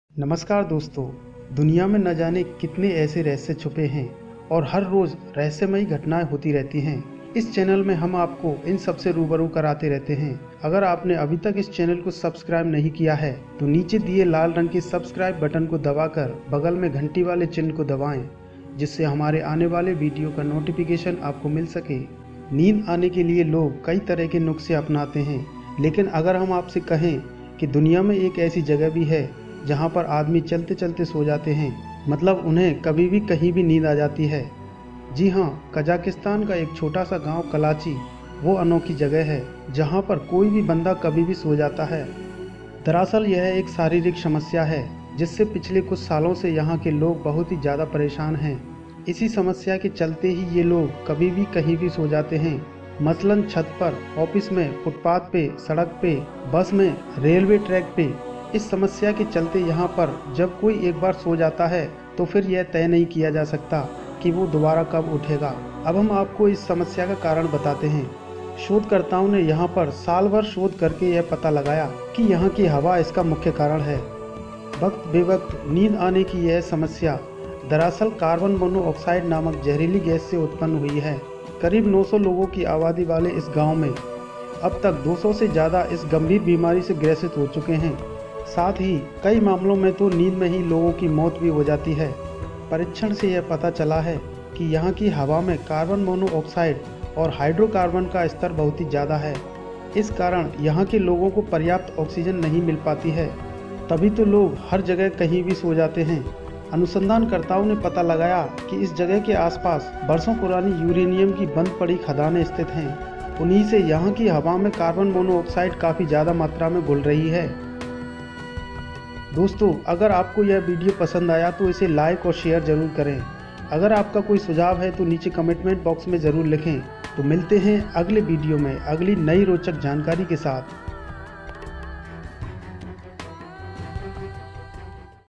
Sleeping town in Kazakhstan | Sleeping mystery | Kalachi village
यहाँ चलते चलते सो जाते हैं लोग